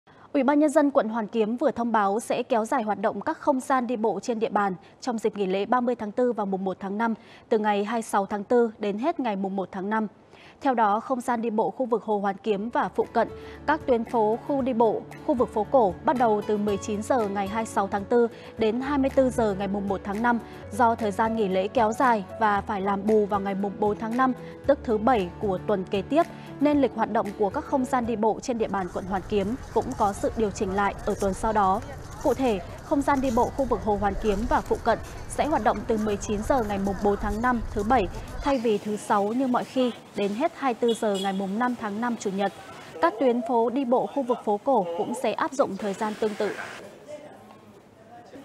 Kéo dài hoạt động phố đi bộ dịp nghỉ lễ 30/4 và 1/5 | Tin tức mới nhất hôm nay
Tối 23-4, quận Hoàn Kiếm (Hà Nội) có thông báo về việc kéo dài hoạt động các không gian đi bộ trên địa bàn quận trong dịp nghỉ lễ kỷ niệm Ngày Giải phóng miền Nam, thống nhất đất nước (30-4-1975 / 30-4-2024) và Ngày Quốc tế Lao động 1-5.